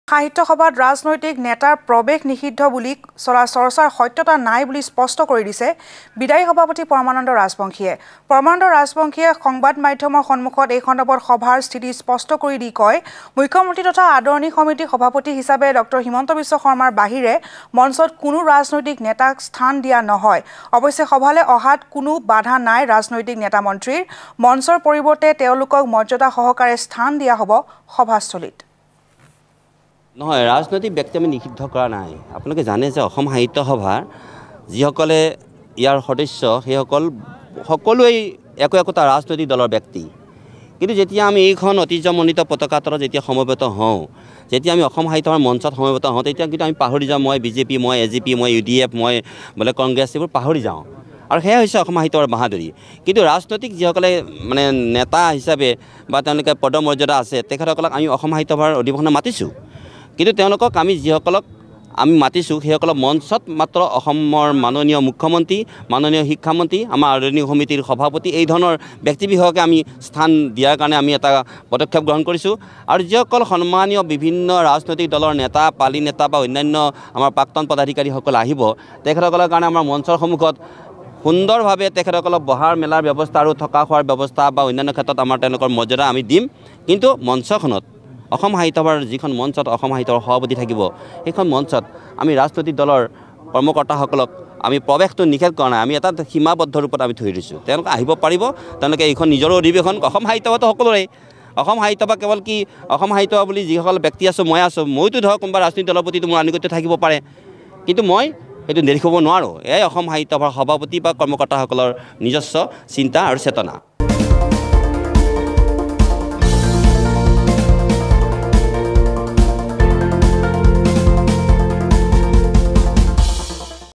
সাহিত্য সভাত ৰাজনৈতিক নেতাৰ প্ৰৱেশ নিষিদ্ধ বুলি চলা চৰ্চাৰ সত্যতা নাই স্পষ্ট সভাপতি পৰমানন্দৰ।
সাহিত্য সভাত ৰাজনৈতিক নেতাৰ প্ৰৱেশ নিষিদ্ধ বুলি চলা চৰ্চাৰ সত্যতা নাই বুলি স্পষ্ট কৰি দিছে বিদায়ী সভাপতি পৰমানন্দ ৰাজবংশীয়ে। পৰমানন্দ ৰাজবংশীয়ে এই সন্দৰ্ভত কয়- মুখ্যমন্ত্ৰী তথা আদৰণি সমিতিৰ সভাপতি হিচাপে ডঃ হিমন্ত বিশ্ব শৰ্মাৰ বাহিৰে মঞ্চত কোনো ৰাজনৈতিক নেতাক স্থান দিয়া নহয়। অৱশ্যে সভালৈ অহাত কোনো বাধা নাই ৰাজনৈতিক নেতা-মন্ত্ৰীৰ।